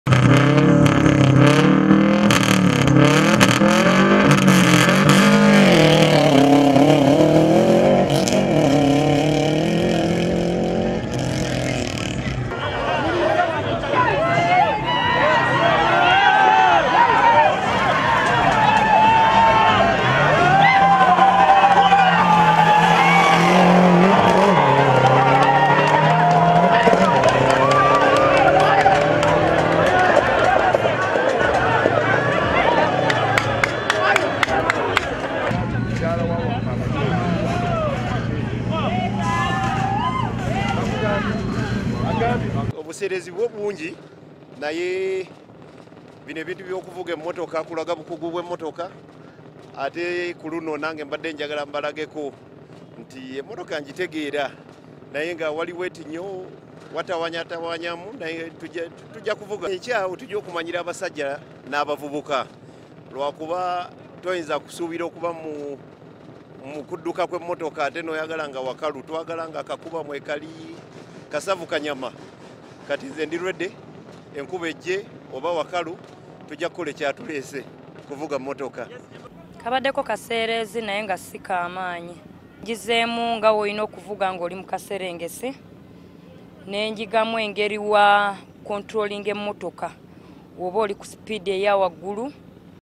Emmotoka z’empaka ez’e Jinja zicamudde abalabi
Abavuzi ba motoka z’empaka bakyamudde bannajinja nga batunka mu mpaka ez’omurundi ogw’okubiri ku kalenda y’omuzannyo guno mu kibuga Jinja.
Motoka asatu mu mwenda zezetabye mu mpaka zino ezitandise olwaleero e Jinja ku Agricultural show ground zikomekerezebwe olunaku lw’enkya e Iganga.
Wabula abavuzi bagamba nti olunaku lw’enkya lwebasinze okwesunga olwamakubo okuba nga gatonyemu enkuba.